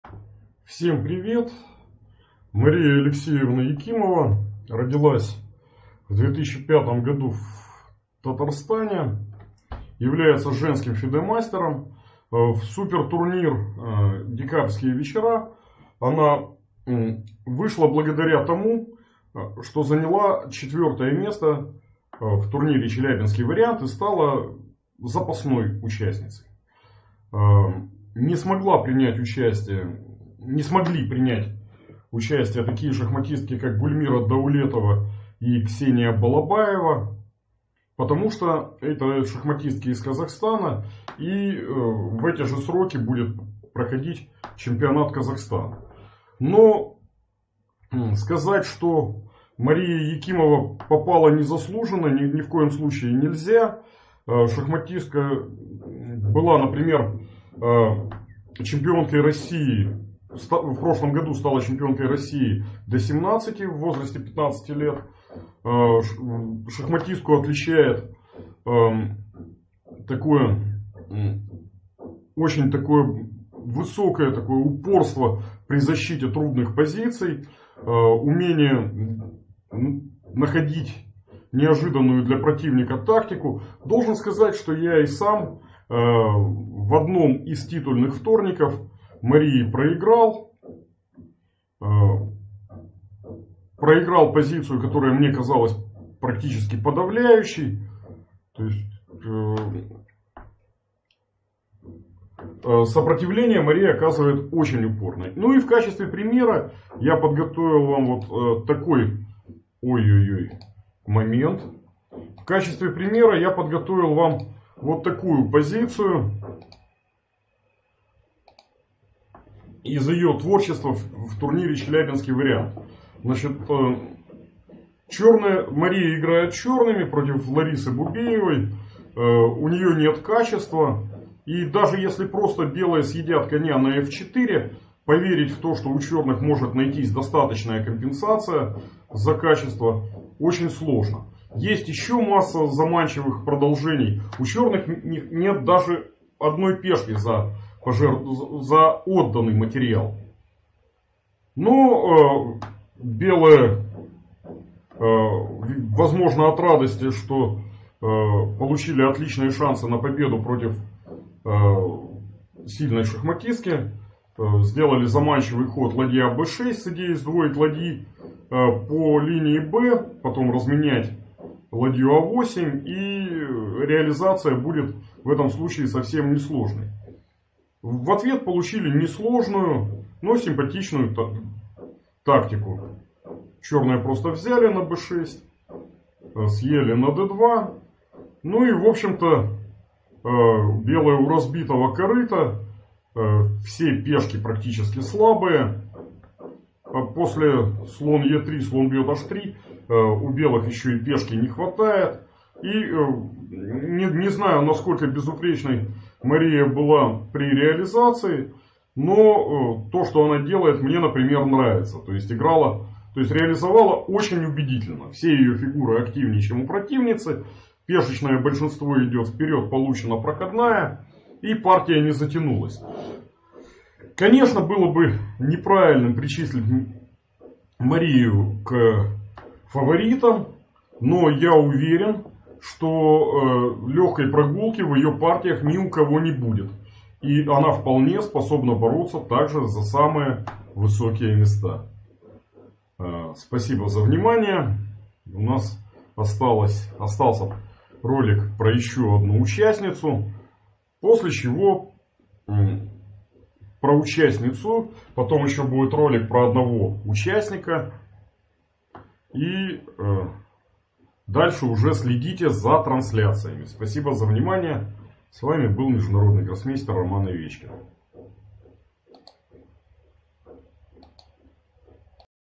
Мария Алексеевна Якимова
Мария Якимова – (17 лет , 2005 г.р., г. Казань) -  с семи лет занимается шахматами и регулярно принимает участие в первенствах России. В 15 лет (2021г) стала победителем первенства России среди девушек до 17.
Планирует связать свою карьеру с шахматами.
 Студентка  «Университета Талантов»
С  шахматами познакомил отец , так как он сам играл в детстве. Когда привели маленькую Машу на первое занятие, оказалось, что она станет ученицей первого тренера папы.
 Марии импонируют шахматистки Хоу Ифань, Екатерина Лагно, Александра Костенюк
Мария : «Как любой спортсмен, я хочу быть известной»
 Упоминаются  как тренеры - Газизов Р.Х.(2018г.), Виктор Туркин (2019 г.)
 Участница Суперфинала чемпионата России по шахматам 2022

"Декабрьские Вечера - 2022"
Турнир по круговой системе среди женщин пройдет в Челябинске с 28 ноября по 8 декабря.

Место проведения - Гранд-Отель "Видгоф", конференц-зал "Консул".

Судейская бригада:
IA, ССВК Евгений Сомкин (главный судья); IA, ССВК Константин Калиев; IA, ССВК Наталья Дружинина; ССВК Ирина Фролова;
ССВК Татьяна Шумякина.
Комментаторы: МГ Роман Овечкин и ММ Алина Бивол.
Фотограф Анна Штурман.
Призовой фонд турнира - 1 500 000 рублей.